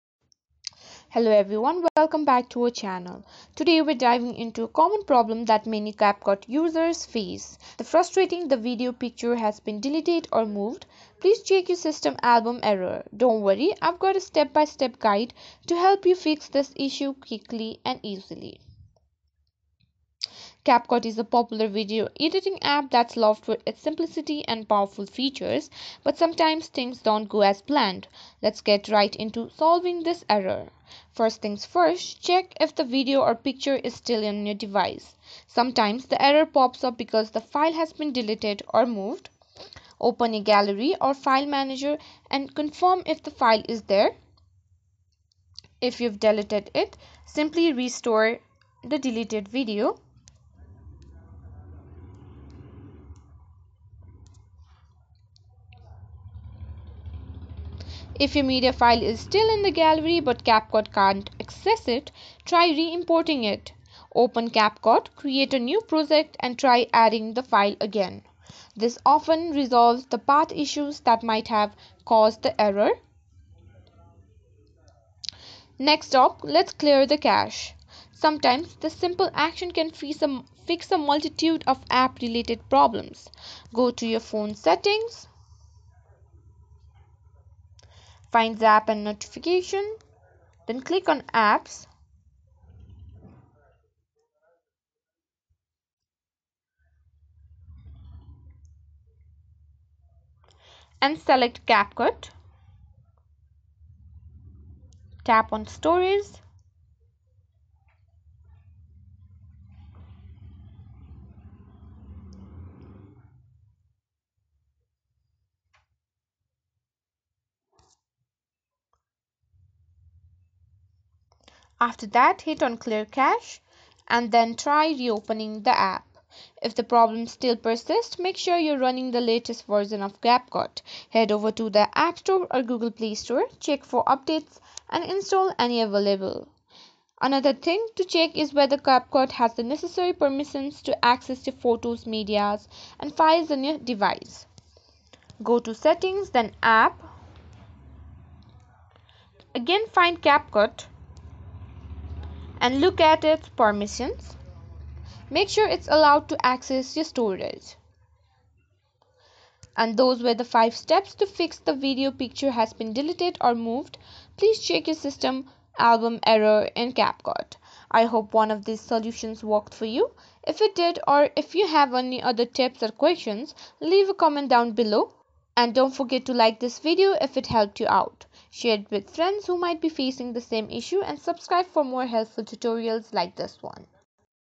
Fix The CapCut video/picture has been deleted or moved please check your system album Problem Solve
Quick Guide to Fix The CapCut video/picture has been deleted or moved please check your system album Problem Solve. Easily fix my CapCut with the help of this tutorial.

Tired of the annoying "video/picture deleted" error in CapCut?  This video from Login Helps will show you exactly how to fix it! We'll explore common causes like accidentally deleted media and outdated apps, then guide you through solutions like checking your recently deleted folder and updating CapCut.